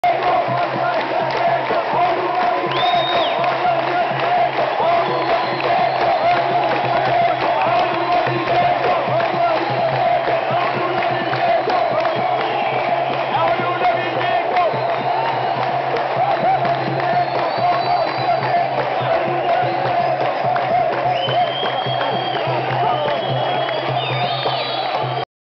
RSB 30 vs 26 CODM MEKNES HANDBALL A BERKANE RSB vs CODM DEBUT DE MATCH
RENAISSANCE SPORTIVE DE BERKANE 30 vs 26 CODM CLUB OMNISPORT DE MEKNES HANDBALL A BERKANE NAHDAT BERKANE Vs MEKNES